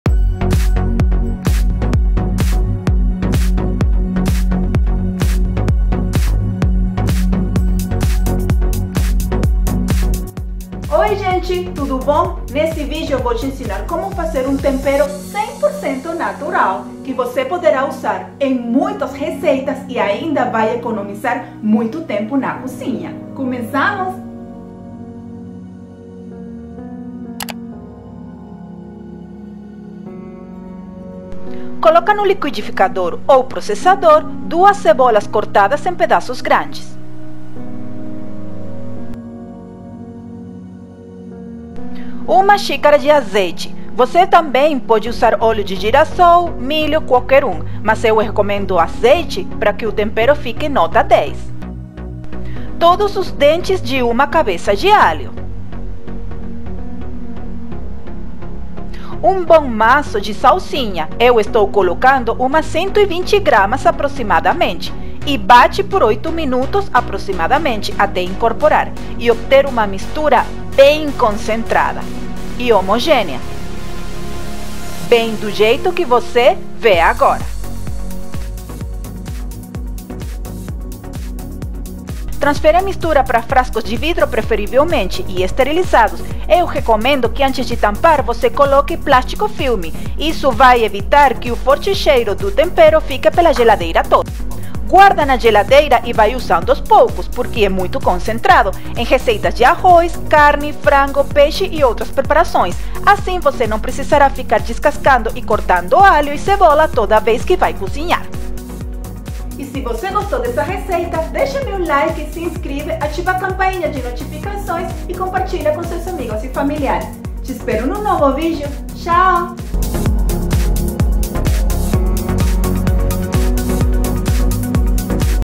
Tempero de Alho, Cebola e Salsinha 100% Natural🧄🧅 Receita fácil e rápida no liquidificador!!! 💯✅🔝🤩
Chega de descascar e cortar alho e cebola toda vez que vai cozinhar!!! Neste video te ensino como fazer um prático Tempero Caseiro 100% Natural, que você poderá fazer com poucos ingredientes e muito fácilmente no liquidificador ou processador!!!

Ingredientes:

✅ 2 cebolas
✅ 1 cabeça de alho
✅ 1 maço de salsinha ou cheiro verde
✅ 1 xícara de azeite, pode usar óleo também, mas eu recomendo azeite para ficar com mais qualidade e melhor sabor.